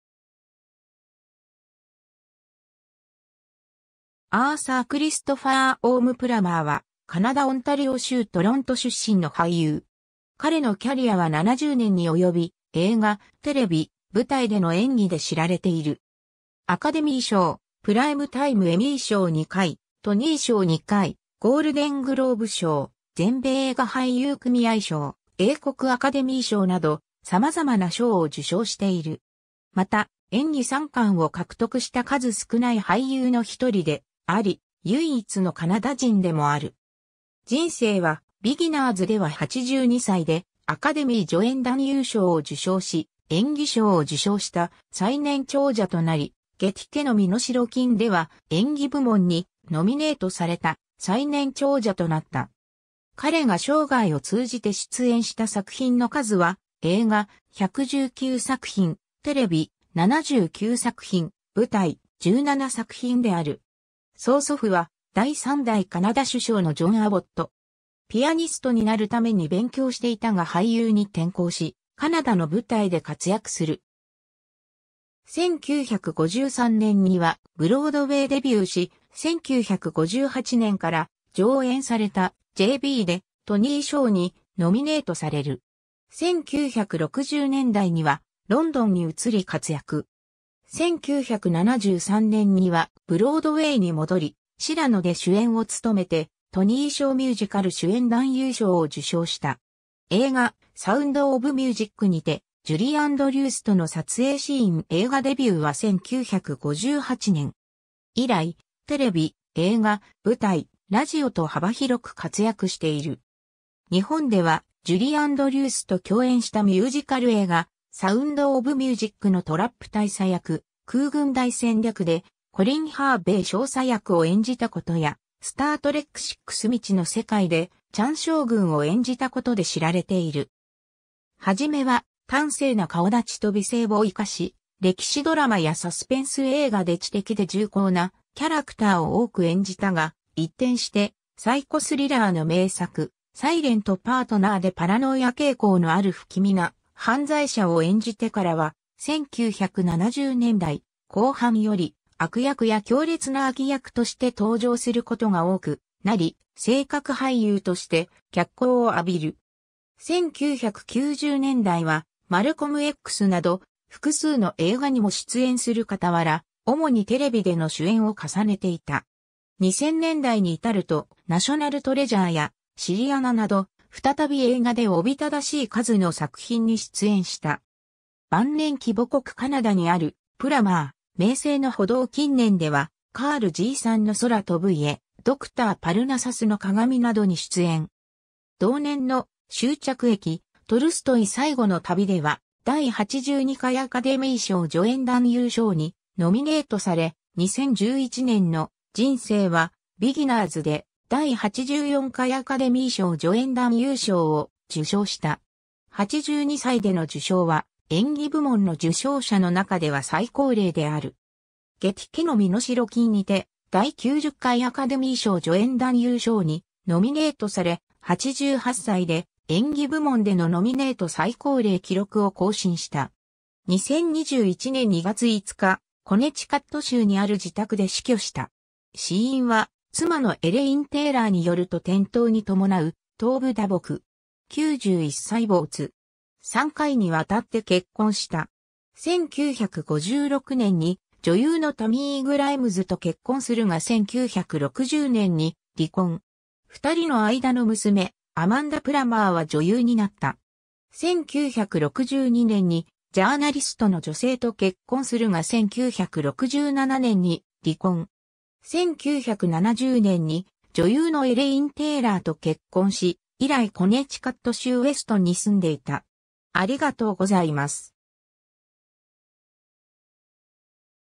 クリストファー・プラマー
アーサー・クリストファー・オーム・プラマー（Arthur Christopher Orme Plummer、1929年12月13日 - 2021年2月5日）は、カナダ・オンタリオ州トロント出身の俳優。
 彼のキャリアは70年に及び、 映画、テレビ、舞台での演技で知られている。
 アカデミー賞、プライムタイム・エミー賞2回、トニー賞2回、ゴールデングローブ賞、全米映画俳優組合賞、英国アカデミー賞など、様々な賞を受賞している。
また、演技三冠（アカデミー賞、エミー賞、トニー賞）を獲得した数少ない俳優の一人であり、唯一のカナダ人でもある。
『人生はビギナーズ』（2010年）では82歳でアカデミー助演男優賞を受賞し、演技賞を受賞した最年長者となり、『ゲティ家の身代金』（88歳）では演技部門にノミネートされた最年長者となった。
彼が生涯を通じて出演した作品の数は、映画: 119作品、テレビ: 79作品、舞台: 17作品である。
 曾祖父は第3代カナダ首相のジョン・アボット（John Abbott）。
ピアニストになるために勉強していたが俳優に転向し、カナダの舞台で活躍する。
1953年にはブロードウェイ・デビューし、1958年から上演された『J.B.』でトニー賞にノミネートされる。
1960年代にはロンドンに移り活躍。
1973年にはブロードウェイに戻り『シラノ』で主演を務めてトニー賞 ミュージカル主演男優賞を受賞した。
 映画『サウンド・オブ・ミュージック』にて、ジュリー・アンドリュースとの撮影シーン（1964年） 映画デビューは1958年。
以来、テレビ・映画・舞台・ラジオと幅広く活躍している。
日本ではジュリー・アンドリュースと共演したミュージカル映画『サウンド・オブ・ミュージック』（1965年）のトラップ大佐役、『空軍大戦略』（1969年）でコリン・ハーベイ少佐役を演じたことや『スタートレックVI 未知の世界』（1991年）でチャン将軍を演じたことで知られている。
 はじめは端整な顔立ちと美声を生かし、歴史ドラマやサスペンス映画で知的で重厚なキャラクターを多く演じたが、一転してサイコスリラーの名作『サイレント・パートナー』でパラノイア傾向のある不気味な犯罪者を演じてからは、1970年代後半より悪役や強烈な脇役として登場することが多くなり性格俳優として脚光を浴びる。
 1990年代は『マルコムX』など複数の映画にも出演する傍ら、主にテレビでの主演を重ねていた。
2000年代に至ると『ナショナル・トレジャー』や『シリアナ』など、再び映画で夥しい数の作品に出演した。
 晩年期（2014年） 母国カナダにあるプラマー「名声の歩道」 近年では『カールじいさんの空飛ぶ家』『Dr.パルナサスの鏡』（どちらも2009年）などに出演。
同年の『終着駅 トルストイ最後の旅』では第82回アカデミー賞助演男優賞にノミネートされ、2011年の『人生はビギナーズ』で第84回アカデミー賞助演男優賞を受賞した。
82歳での受賞は、演技部門の受賞者の中では最高齢である。
 『ゲティ家の身代金』にて第90回アカデミー賞助演男優賞にノミネートされ、88歳で演技部門でのノミネート最高齢記録を更新した。
 2021年2月5日、コネチカット州にある自宅で死去した。
死因は、妻のエレイン・テイラーによると転倒に伴う頭部打撲。
91歳没。
 3回にわたって結婚した。
1956年に女優のタミー・グライムズと結婚するが1960年に離婚。
2人の間の娘アマンダ・プラマーは女優になった。
1962年にジャーナリストの女性と結婚するが1967年に離婚。
1970年に女優のエレイン・テイラーと結婚し、以来コネチカット州ウェストンに住んでいた。